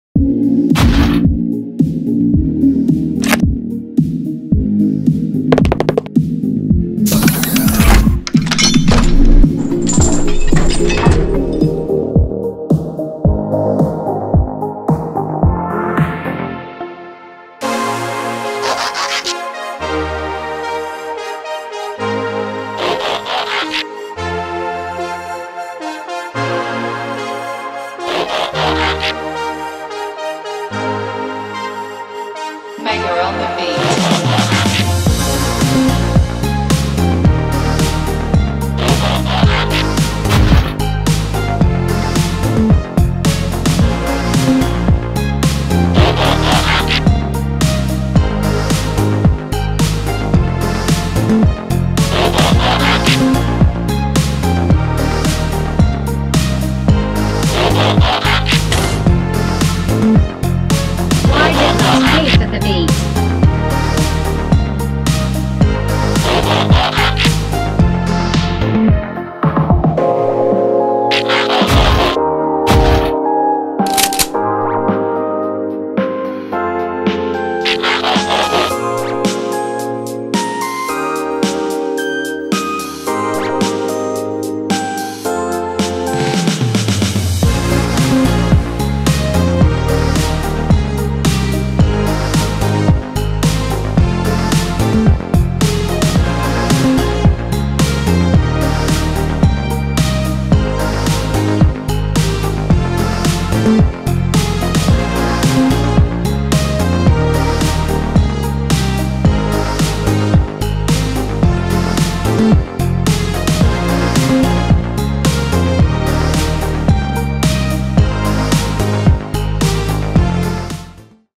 Merry Christmas！Soundwave and his new Tapes~ Transformers stop motion animation By Mangmotion
Merry Christmas all my friends！Thank you for this year's supporting！
52toys fantasy box Santa Claus
Robot Paradise RP-01B Ravage, Rumble & Laserbeak Set of 3
and Masterpieace 15/16 G1 Ravage/Laserbeak